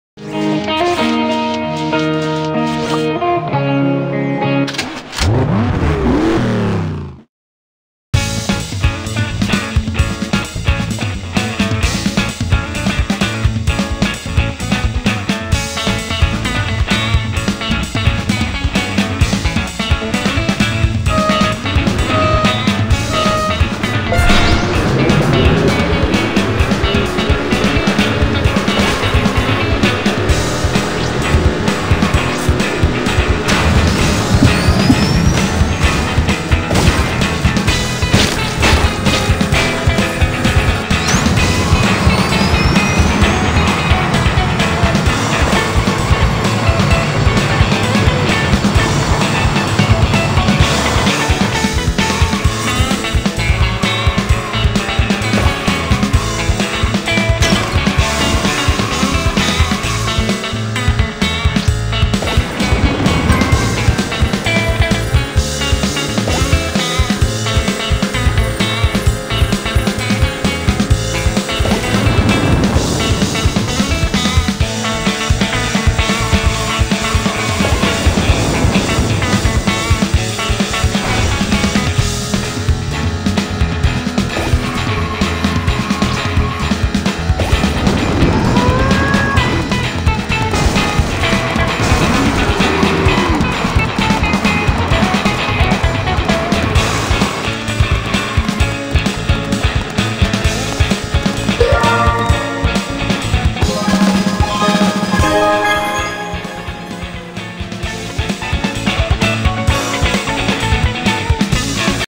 BeatBot with Sarge in Sun City Strip
Watch the races with your friends and score points by guessing from 1-6, the final position of the main racer.
For the love of the game. No cheating.